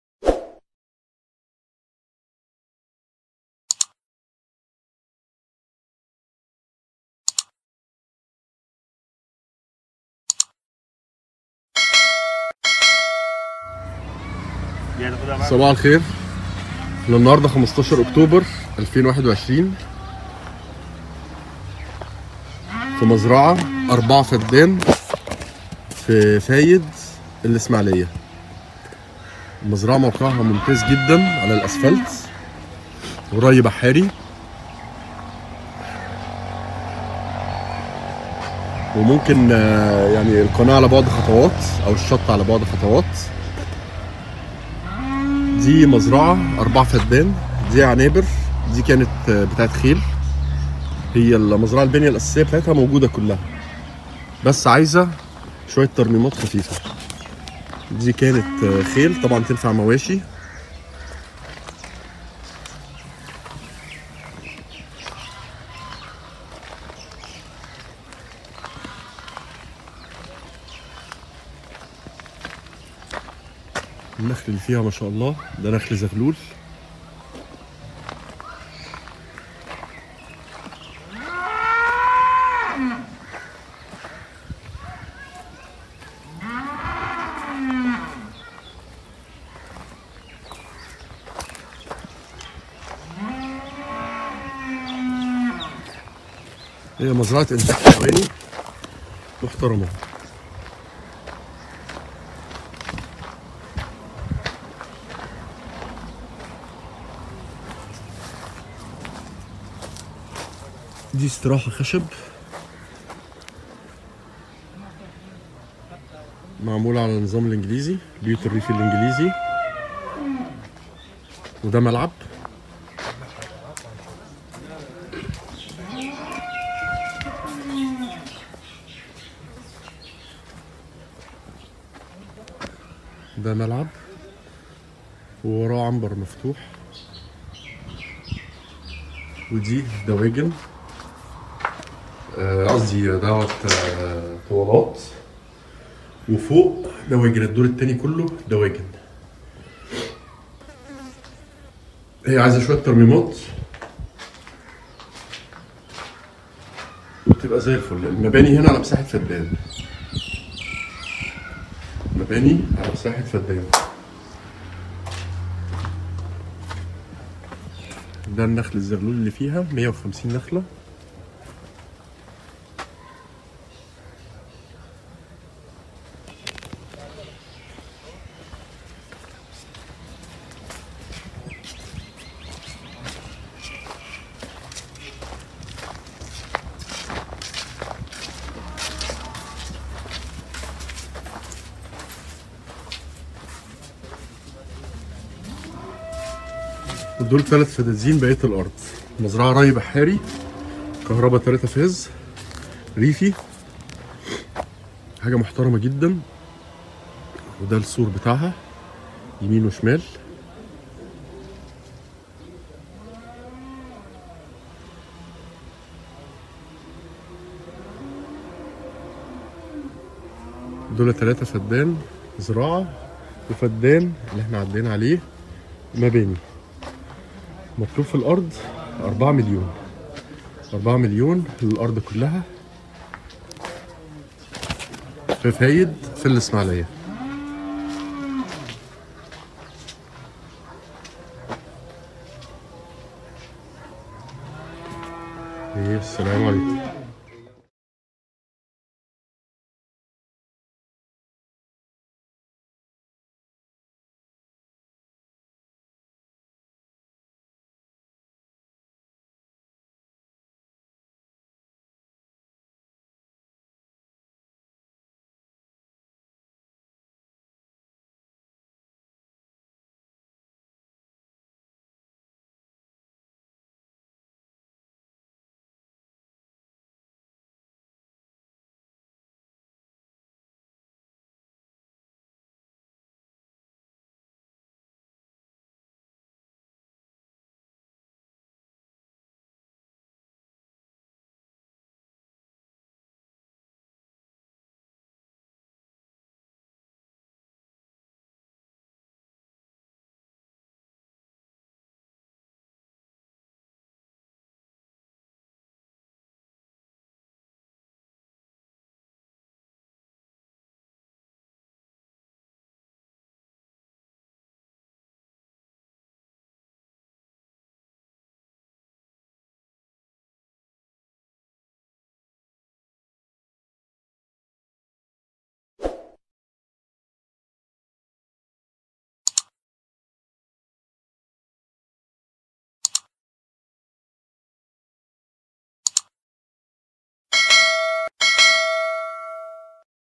CLP مزرعة للبيع رانش خيل و مواشي و دواجن 4 فدان علي قناة السويس الاسماعيلية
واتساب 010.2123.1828
عقد ملك نهائي خالص الثمن ليس عليها جنيه لاي جهة
اسطبلات خيل و عنبر مواشي و عنبر دواجن و بيت ريفي طابع انجليزي
ري بحاري – كهرباء ريفي
التفاصيل كاملة بالفيديو
مطلوب 4 مليون جنيه مصري
لدينا العديد من المزارع مختلف المساحات بدءا من 5 فدان الي اكثر من 50 الف فدان
لو عندك مزرعة مميزة و عايز تبيعها كلمنا و سيب الباقي علينا